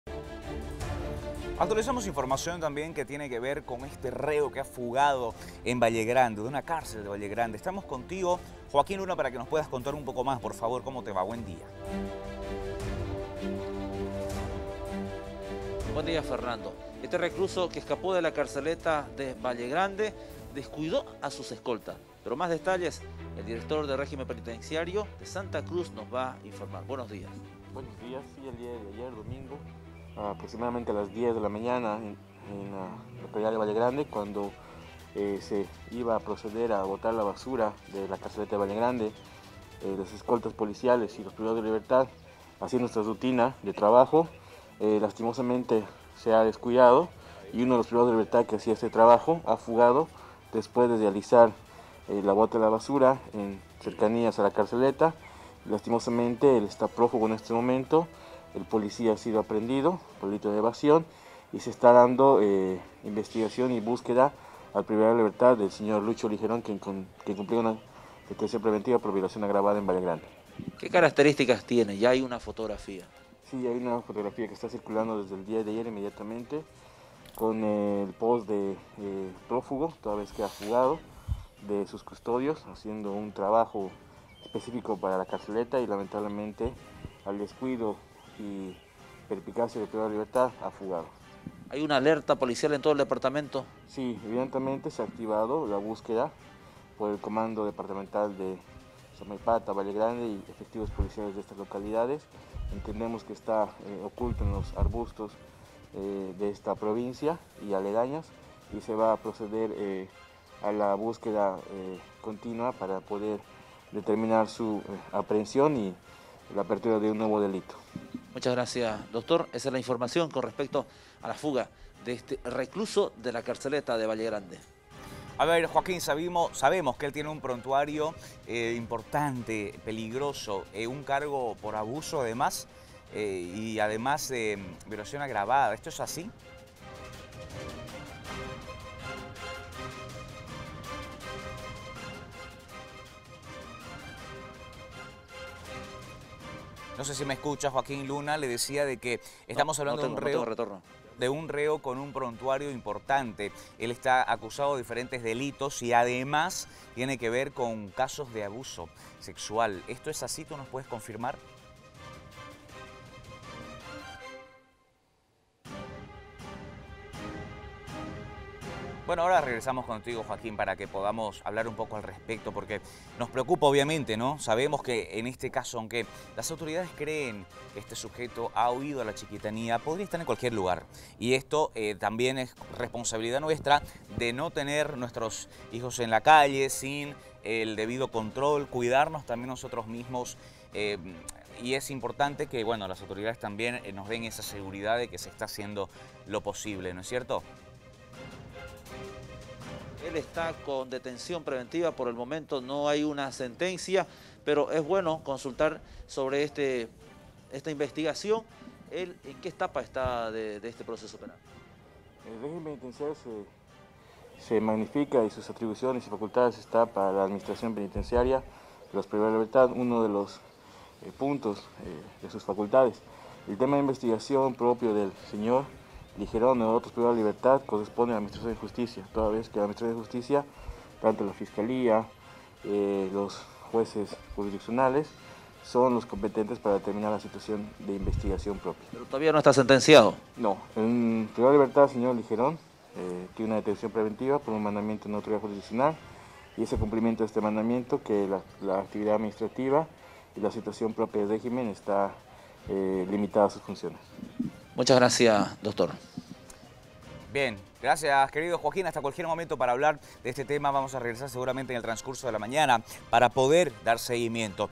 Se fuga un reo del penal de Vallegrande
Las autoridades de régimen penitenciario informaron de la fuga del interno Lucho Lijeron Arancibia desde la carceleta de Vallegrande, el reo fugado es acusado de la presunta comisión del delito de violación agravada. La policía hace rastrillaje en los pueblos de la Chiquitanía donde inicialmente se cree que se esconde intentado huir al Brasil.